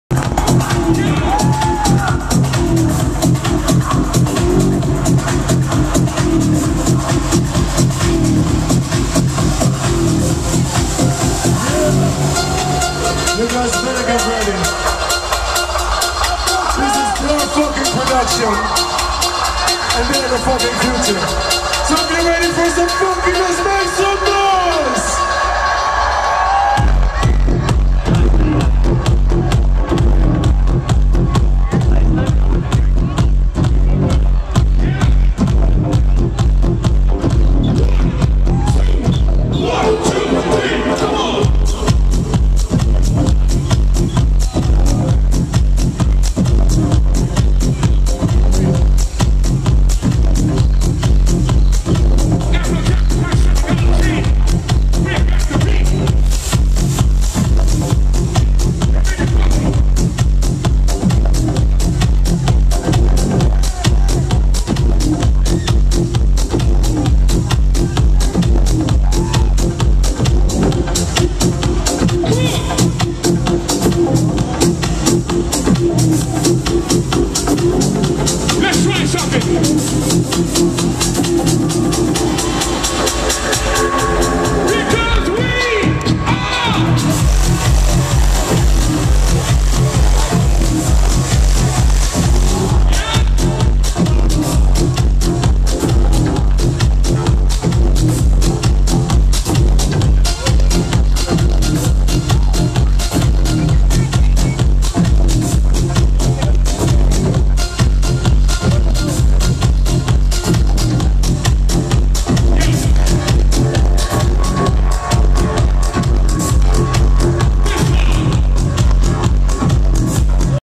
Afrojack Live - Ultra Music Festival 2011
This is  Afrojack's third SURPRISE performance at the Main stage at ULTRA MUSIC FESTIVAL 2011

for pictures and more on this event!!